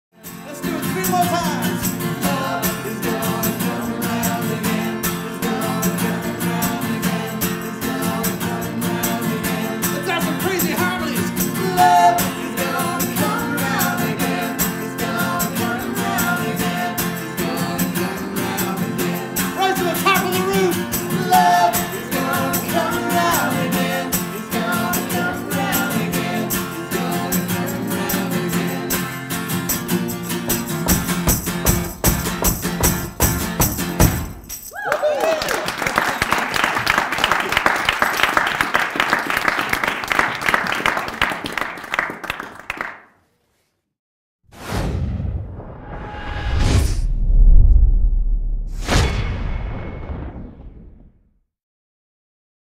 Home Routes Convert Series: The Young Novelists
CHET TV and the Home Routes Concert Series presents The Young Novelists performing at the Dawson Creek Art Gallery.